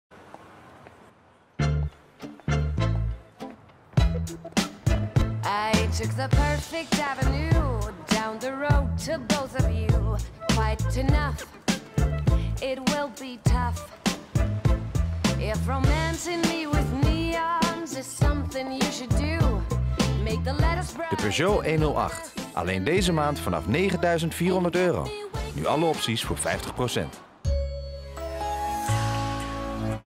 Peugeot 108: al voor €9.400,- / Alleen deze zomermaand!
Kies uw voordeel op de Peugeot 108. In de maand juli kunt u kiezen voor nog meer Peugeot in uw Peugeot 108. U profiteert deze zomer van 50% voordeel op álle opties en een inruilpremie van € 1.000.
En op een model uit onze voorraad profiteert u t/m 31 juli van € 500 voorraadpremie waardoor u al vanaf € 9.400 in een nieuwe Peugeot 108 rijdt. Kom snel naar onze showroom en informeer naar uw voordeel. Of kijk voor meer sterke zomerse prijzen bij Nefkens op